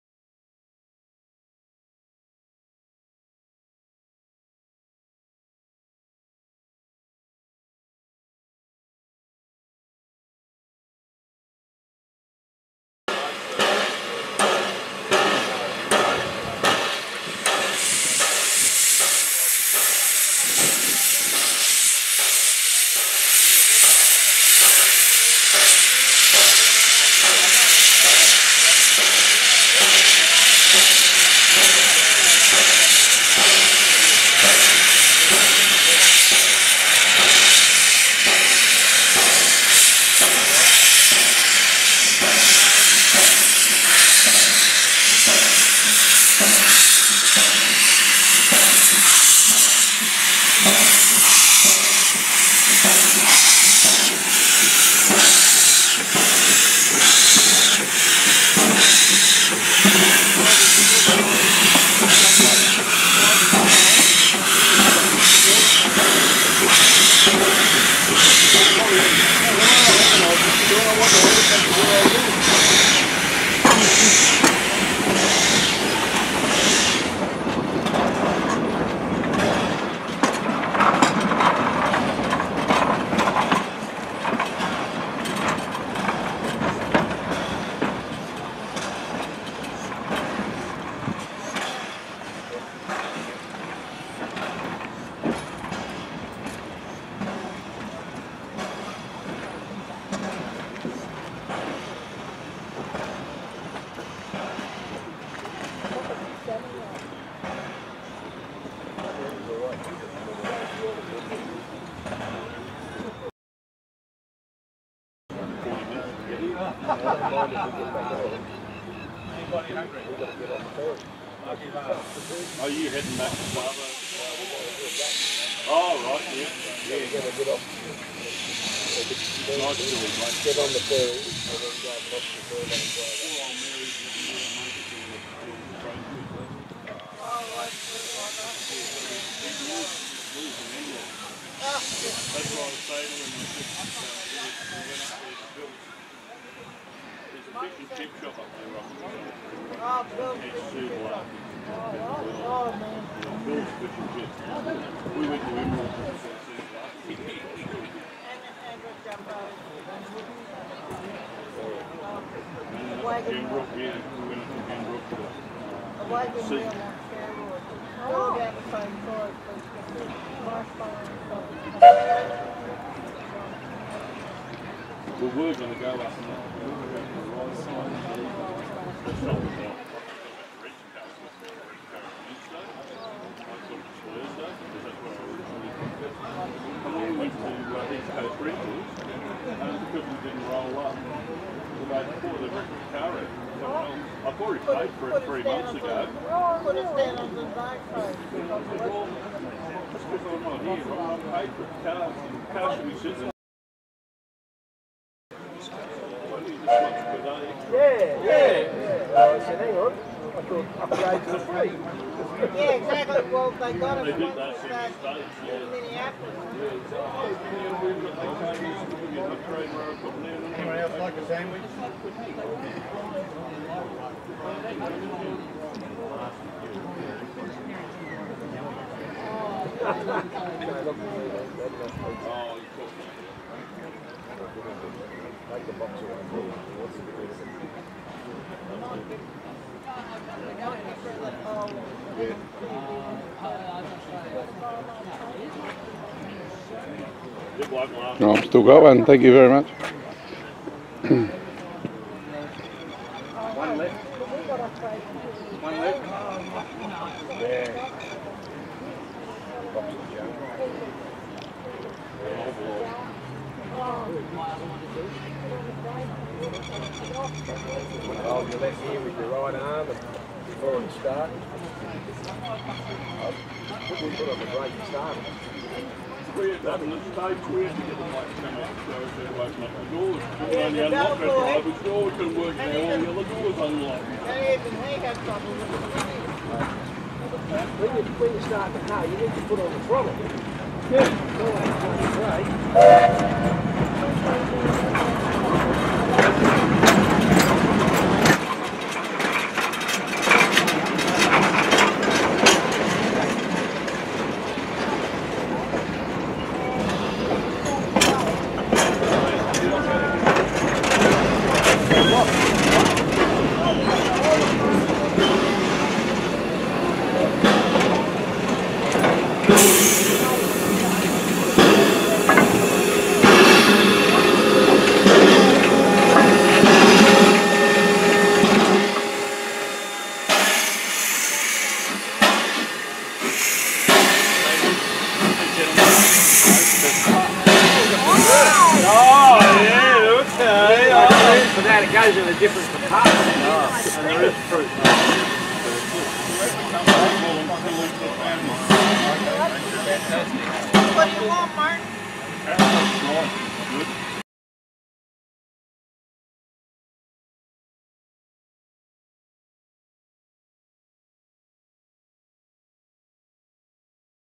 The Bellarine Railway Queenscliff Shunt to Platform
As part of the Australian Narrow Gauge Convention 2017 on 17th April 2017 a chartered steam train on the Bellarine Railway took enthusiasts on a trip to Queenscliff.  After the normal service departed out train was shunted to the platform.